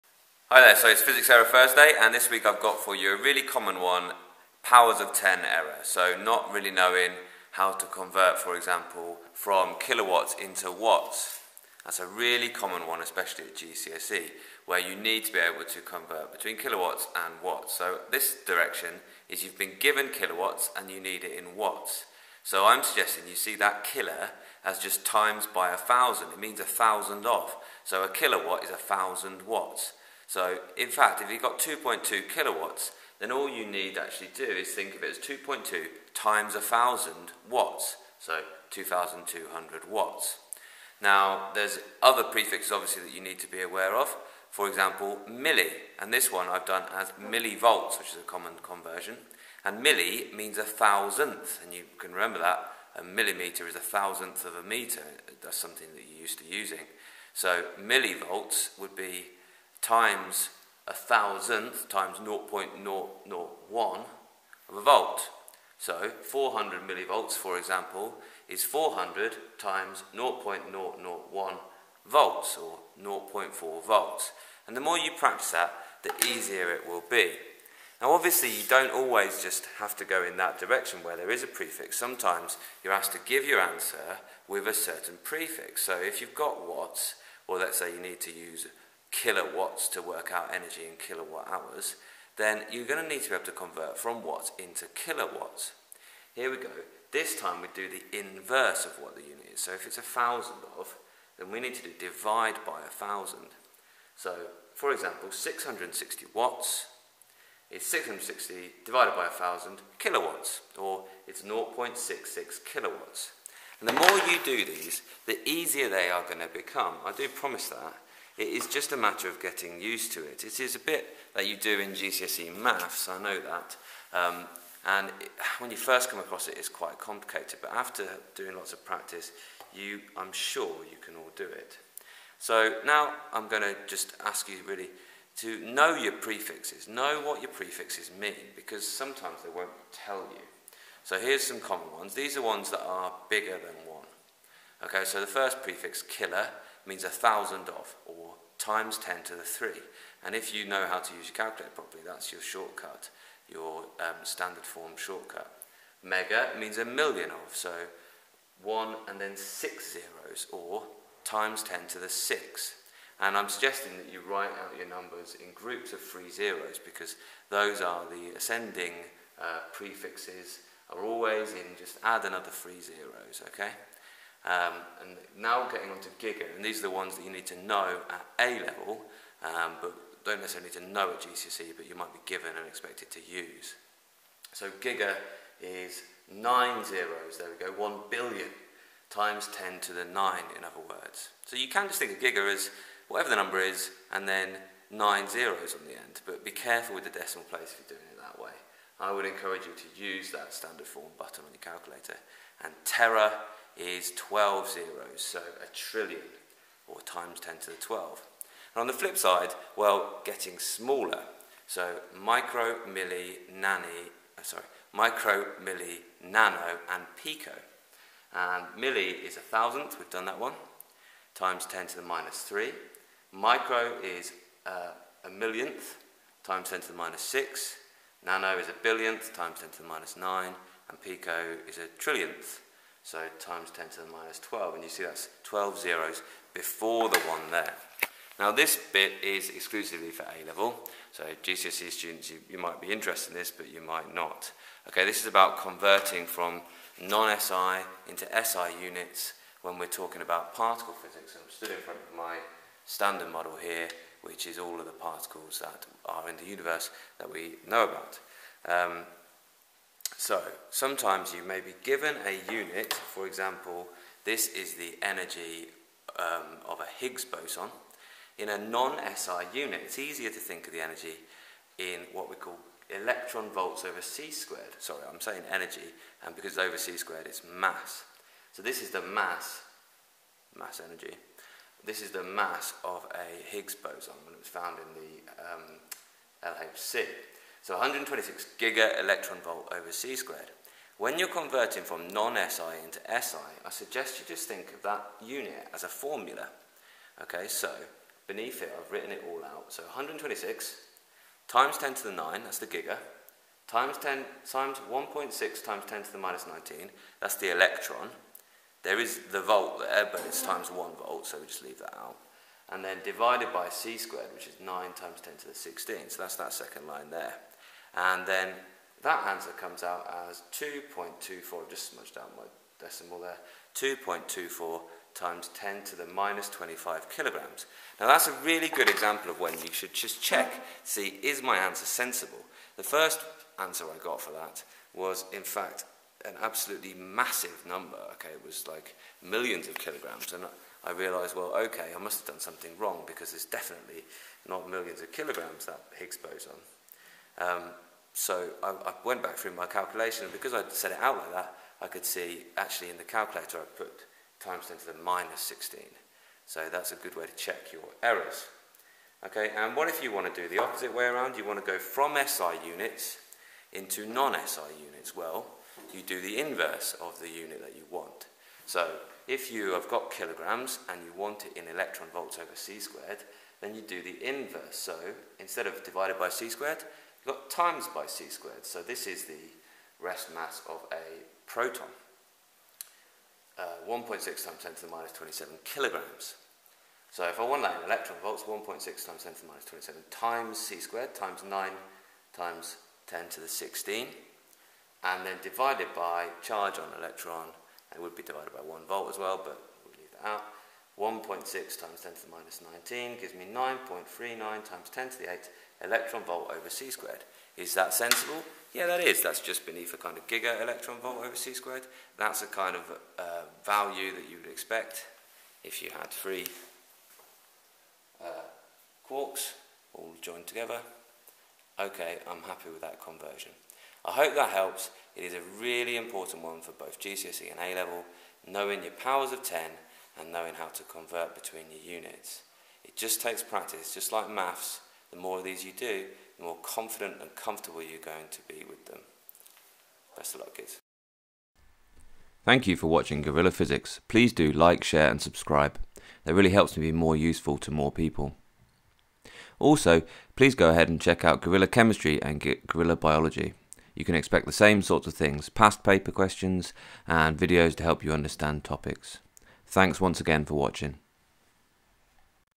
Physics Error Thursday [5] - Powers of Ten Error - GCSE and A Level Physics
This is an error I see quite a lot, it could be as simple as missing a zero off in the calculator, or converting the wrong way around!  You can avoid this error with a bit of practice and some good discipline with your numeracy skills!